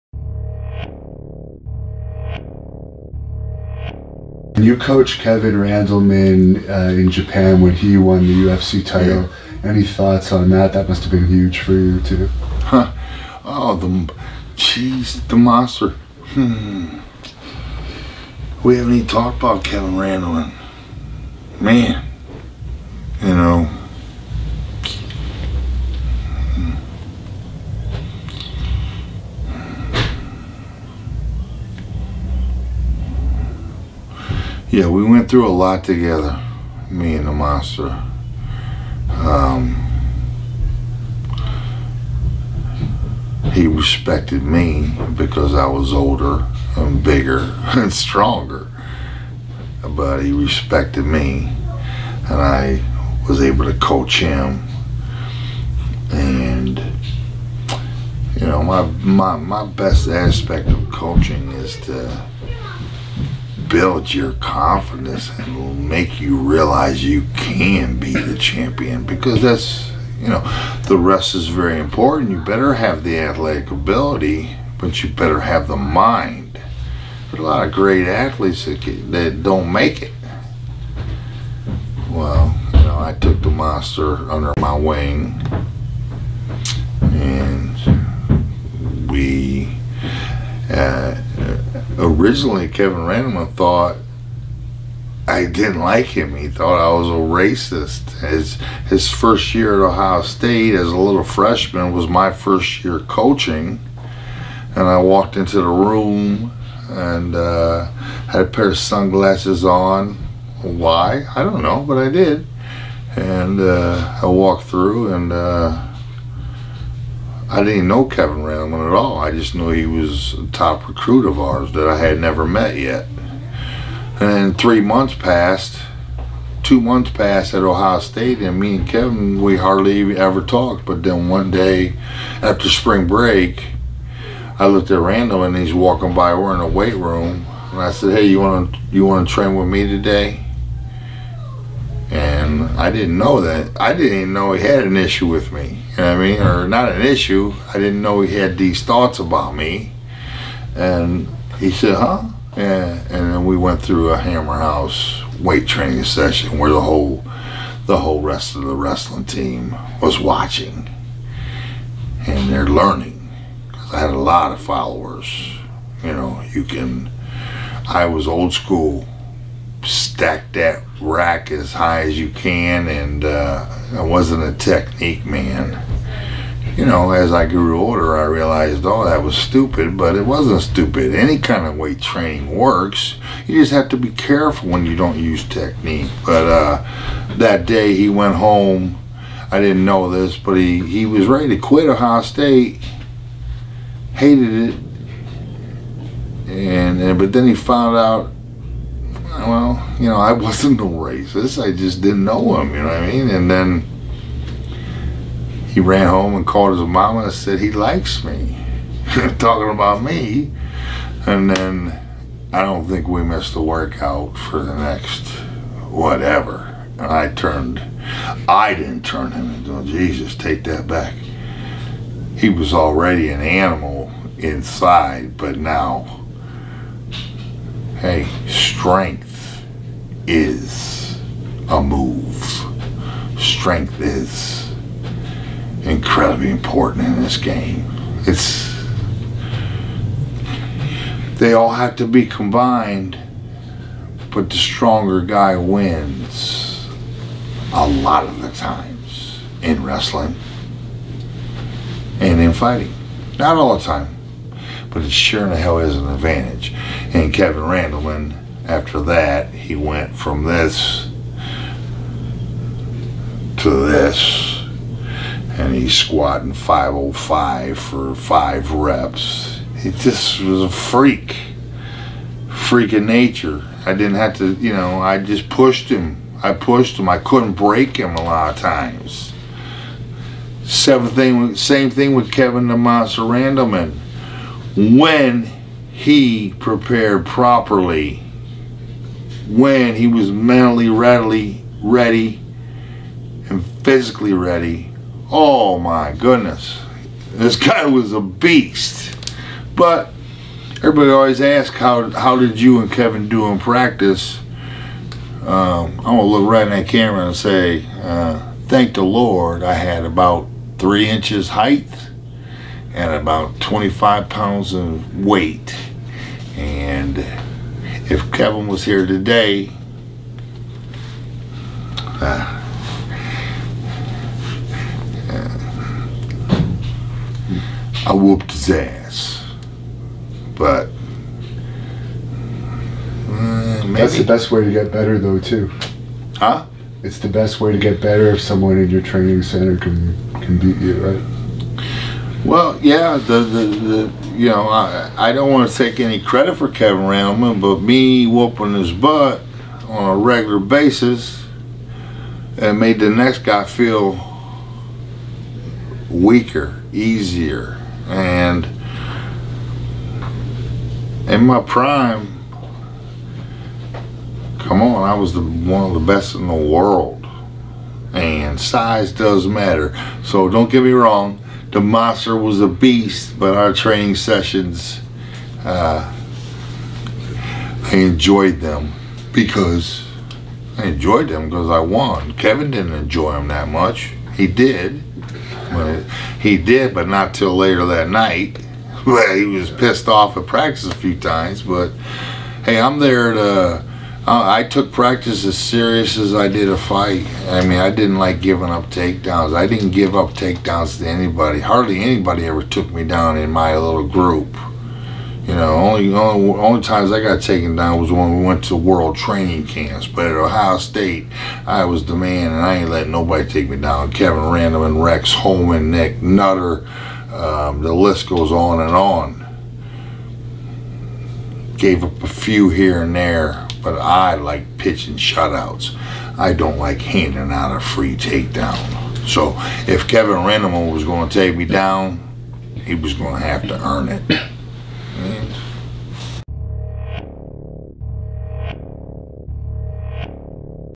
Mark Coleman on Kevin Randleman
Ultimate Fighting Championship Hall of Famer & 1st ever UFC Champion Mark "The Hammer" Coleman talks "Team Hammer House" member Kevin Randleman who Mark coached since their days in Ohio State training together with weights and on the mats.